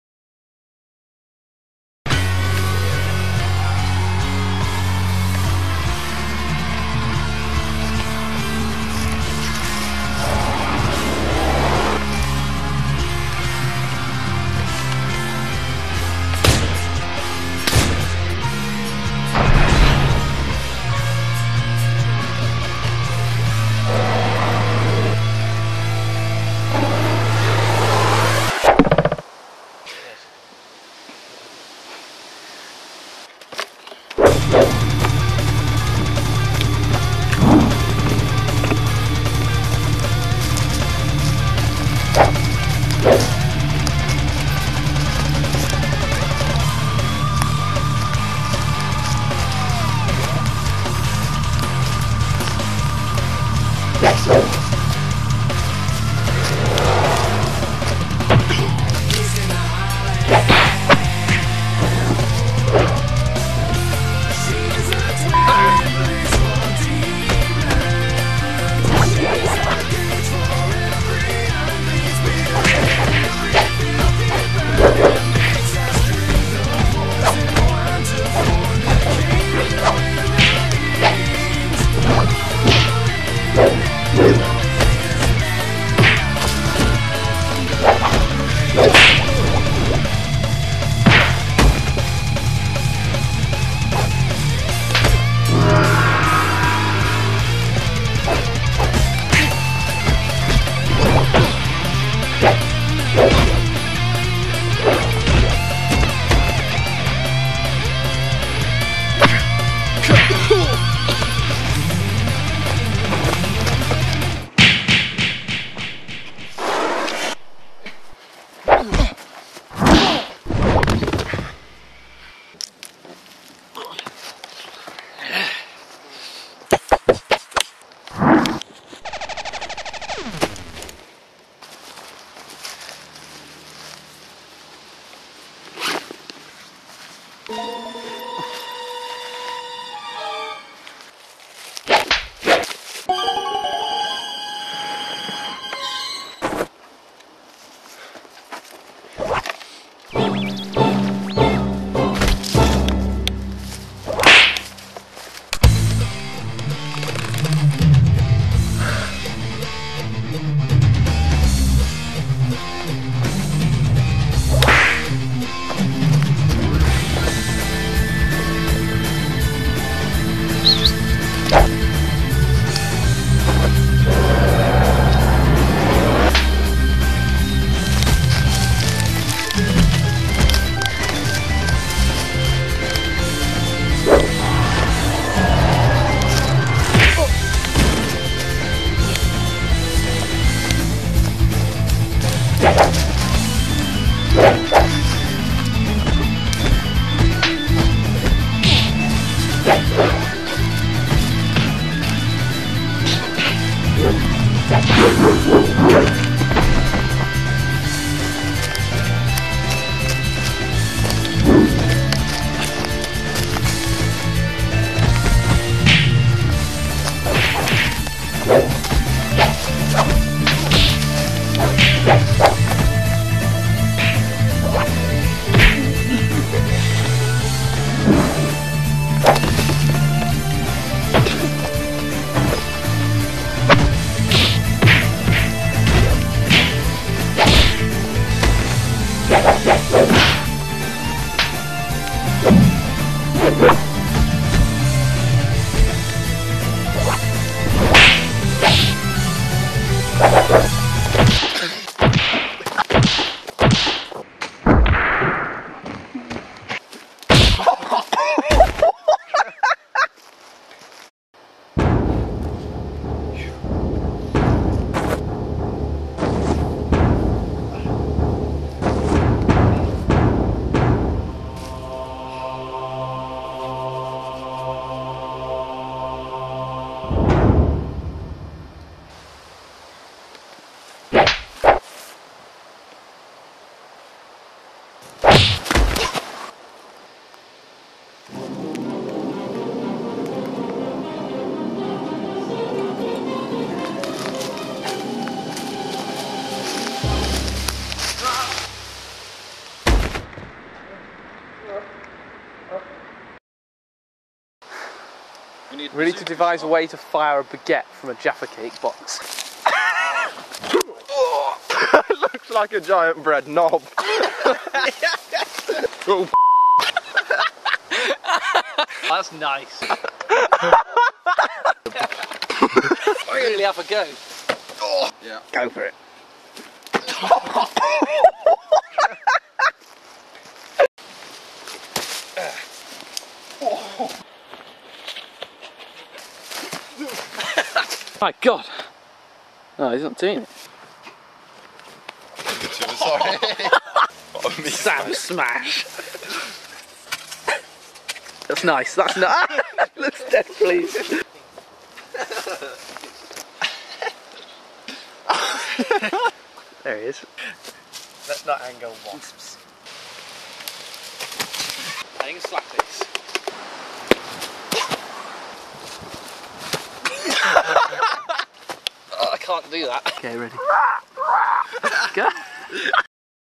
Yorkie Brawl 3 - Ninja Breadzooka
The fairly ninja threequel (oh, yes) to the classic Yorkie Brawl and Minty Hoops.

Features the latest in non-lethal ordnance, flipping out, and a real-life fisherman's hook.